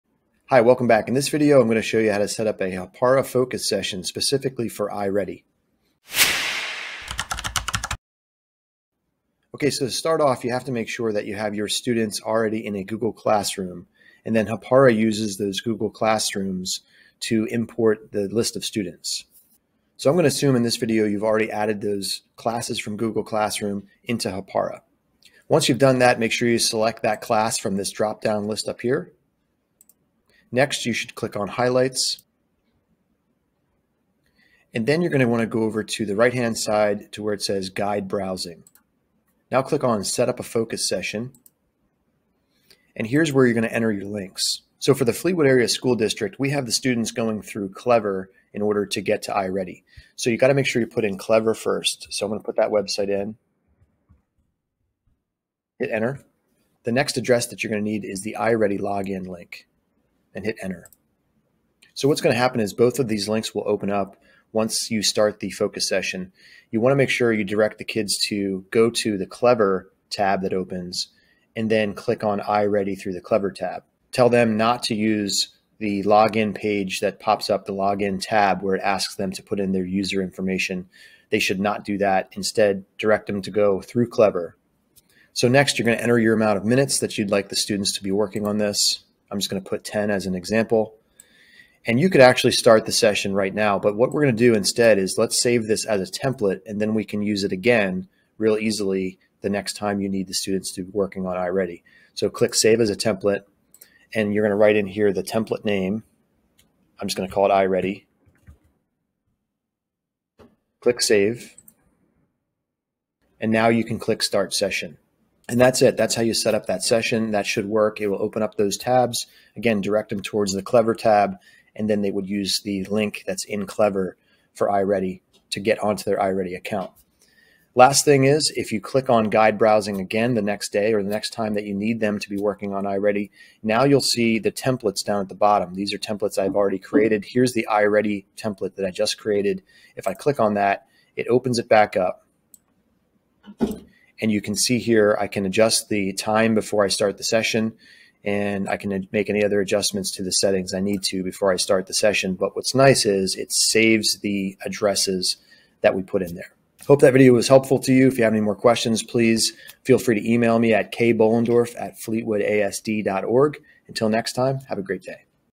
Setting Up a Hapara Focus Session for i-Ready and Clever
How to set up a Hapara focus session for i-Ready and Clever.